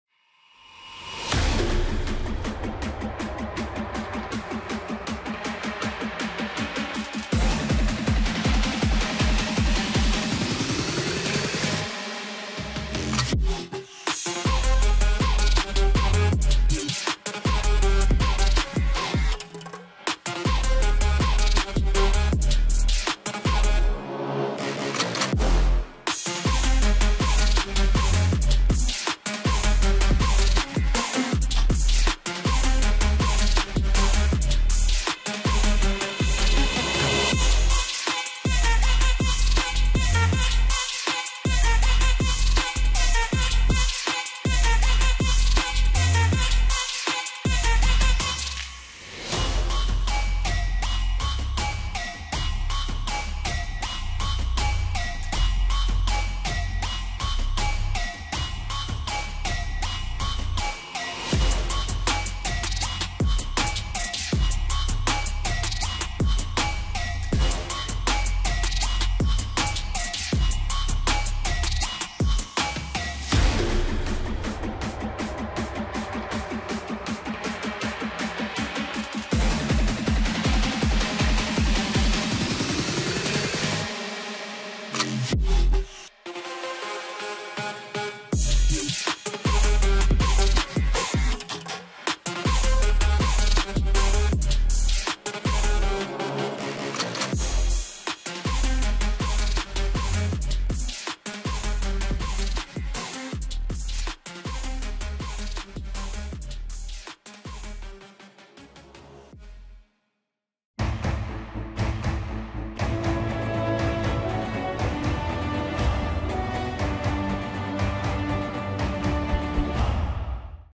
LaLiga Best Assists: Dani Alves, Denis Suárez & Budimir
¡Disfruta de las mejores asistencias de la jornada 35 de LaLiga Santander 2021/2022!